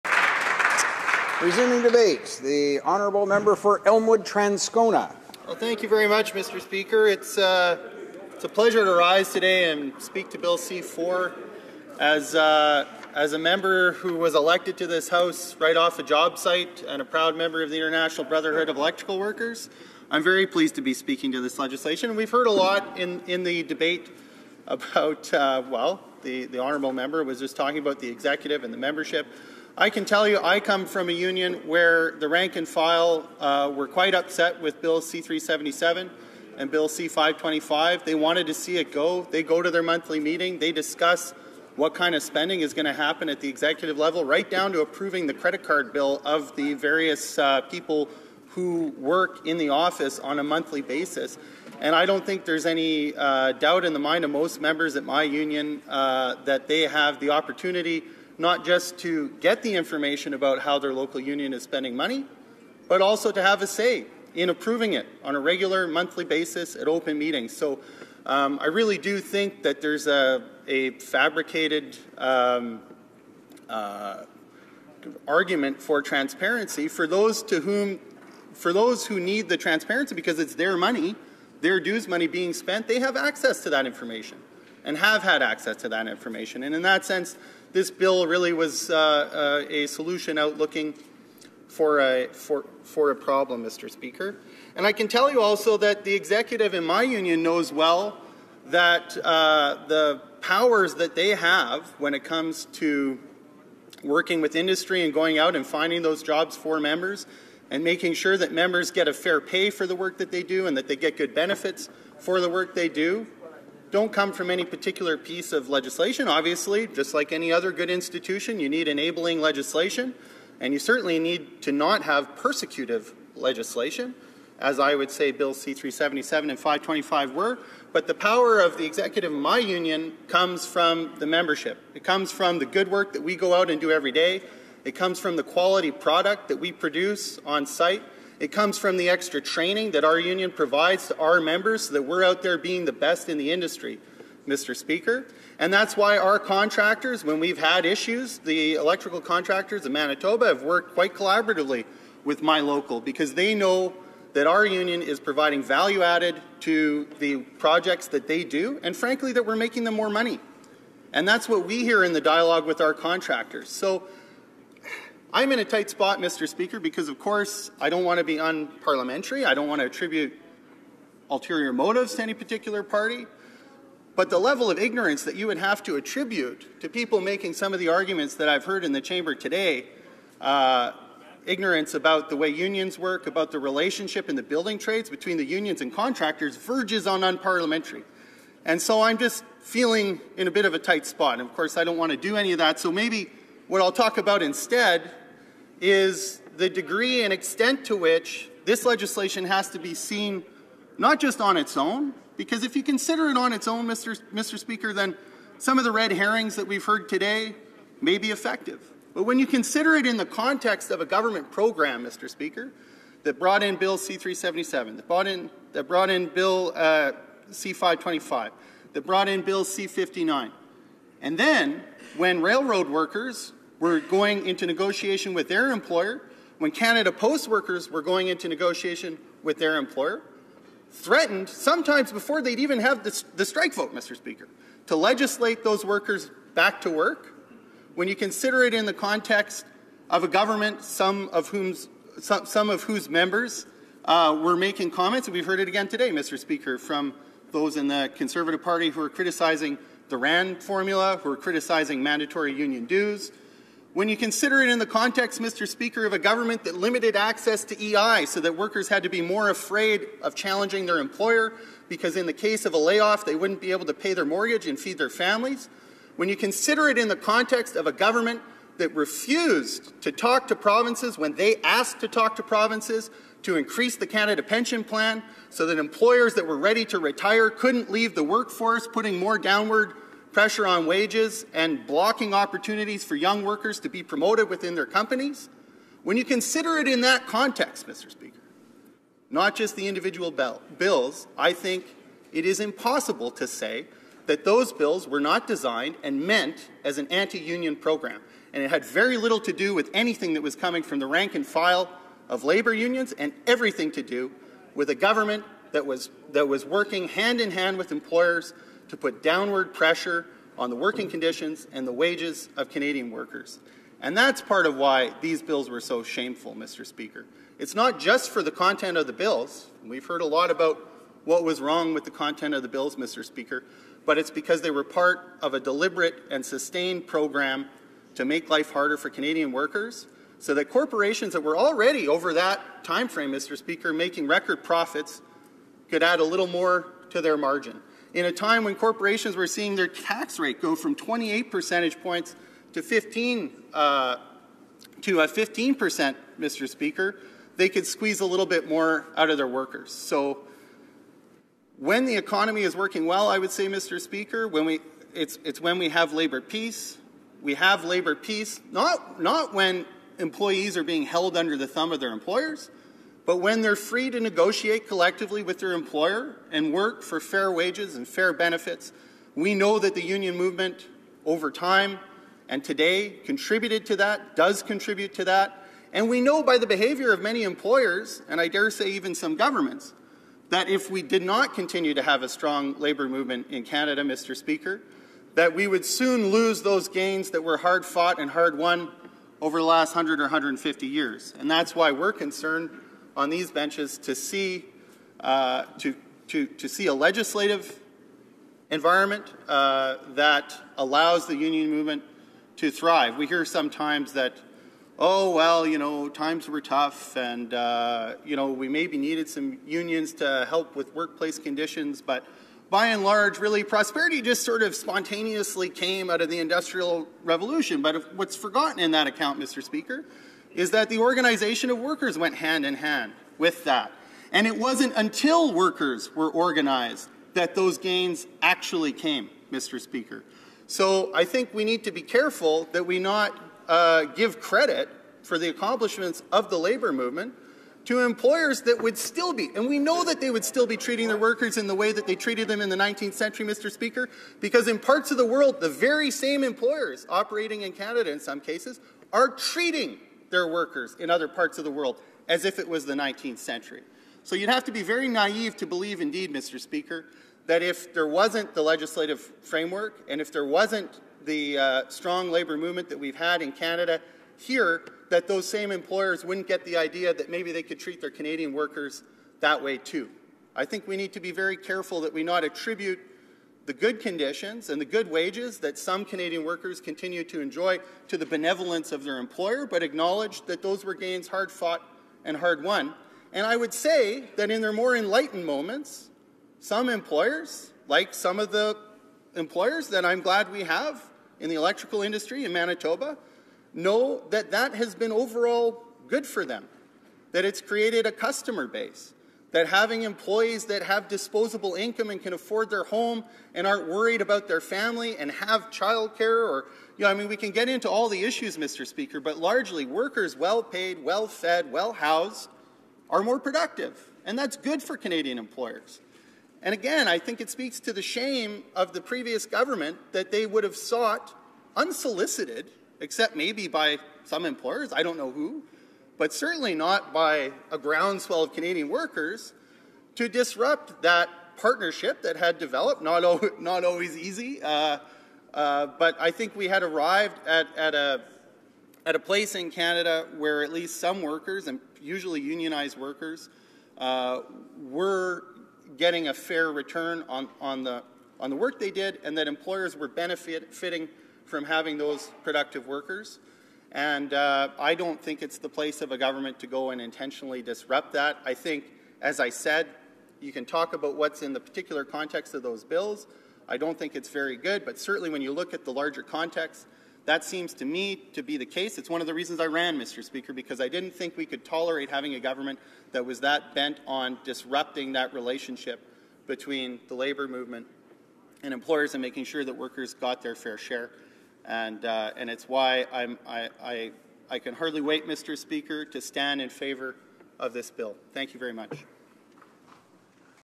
The Importance of the Canadian Labour Movement: Daniel`s speech on the repeal of C-377 and C-525
Daniel speaks in the House of Commons on Bill C-4, An Act to Amend the Canada Labour Code, on February 16th, 2016